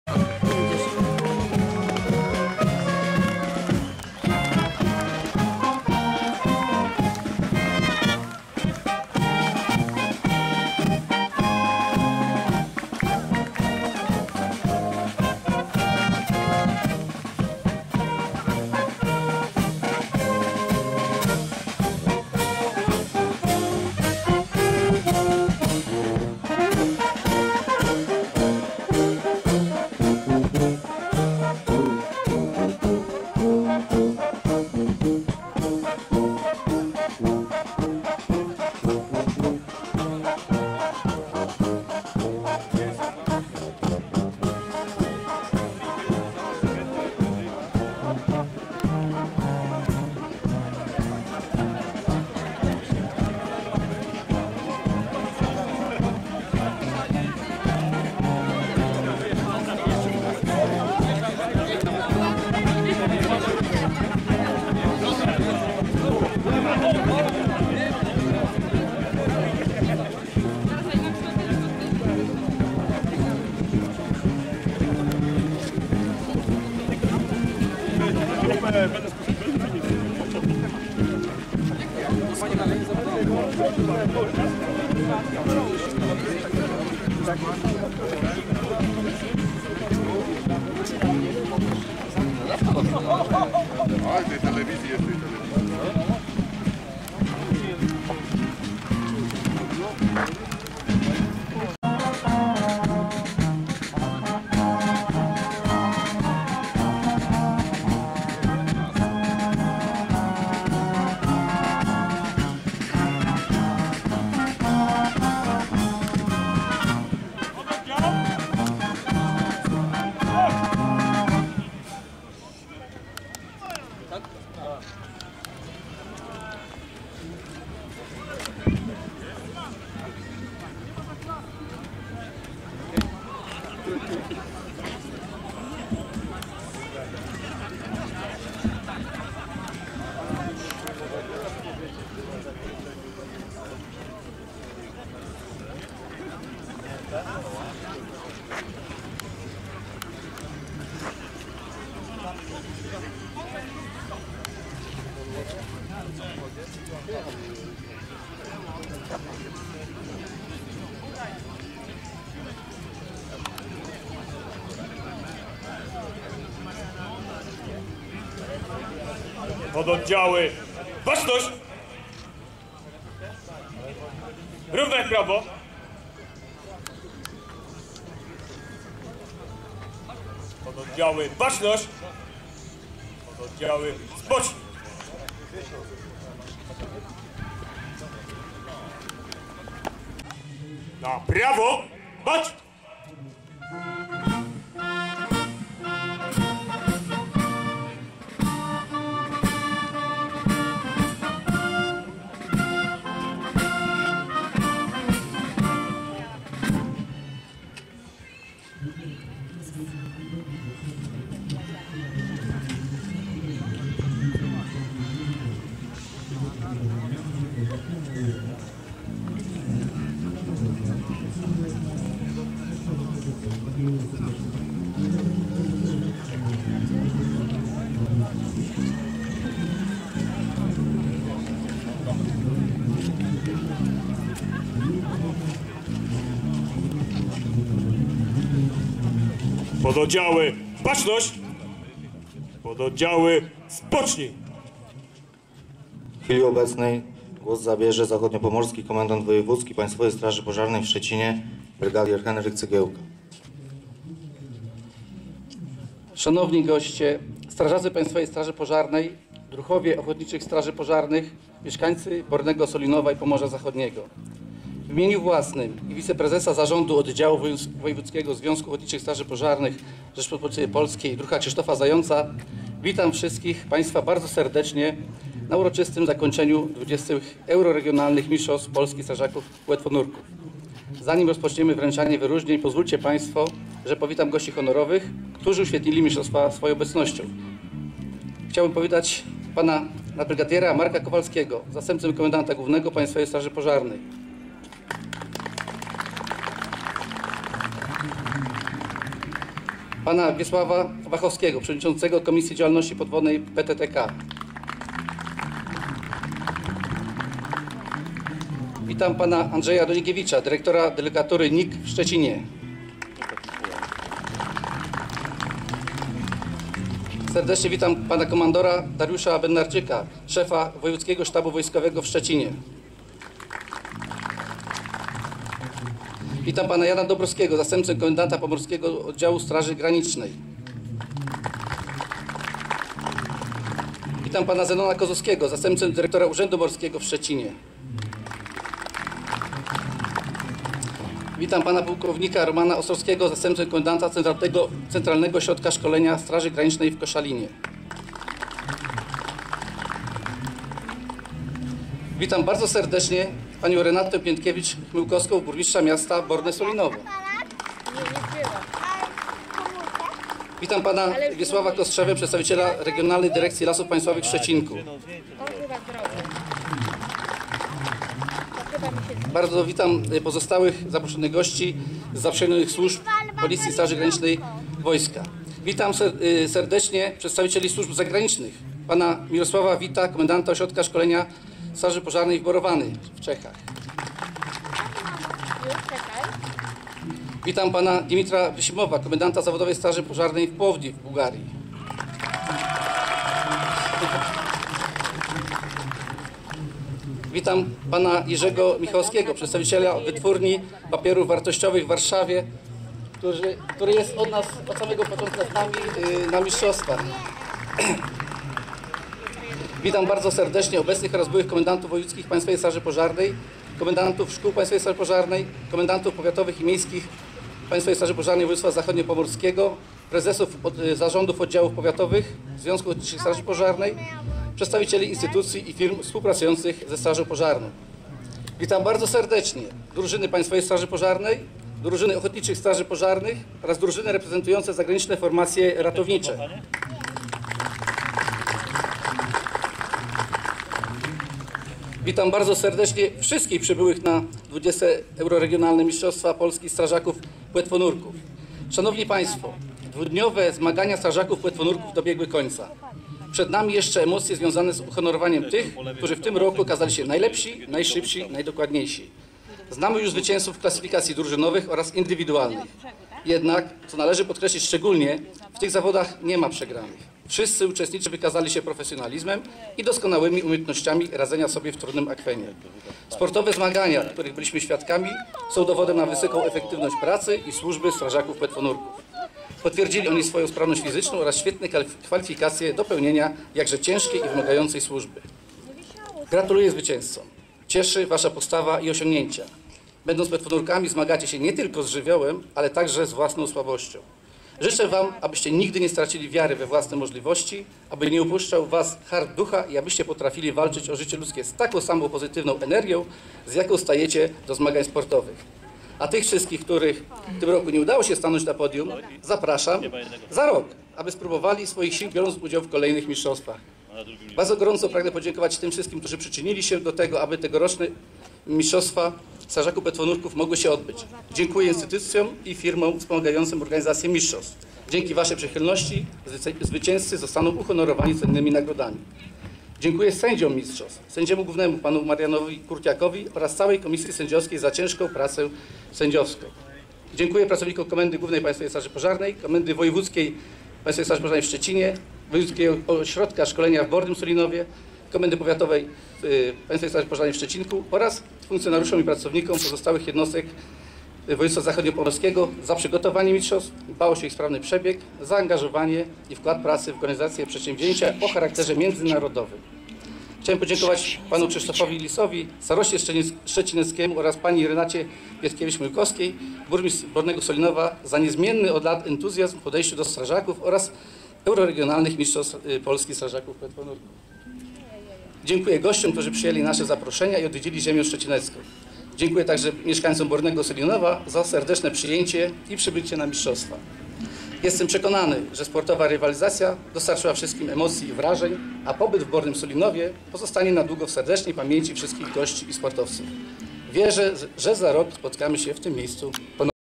Euroregionalne Mistrzostwa Strażaków Płetwonurków
Część 1 oficjalnego zakończenia Euroregionalnych Mistrzostw Strażaków Płetwonurków w Bornem Sulinowie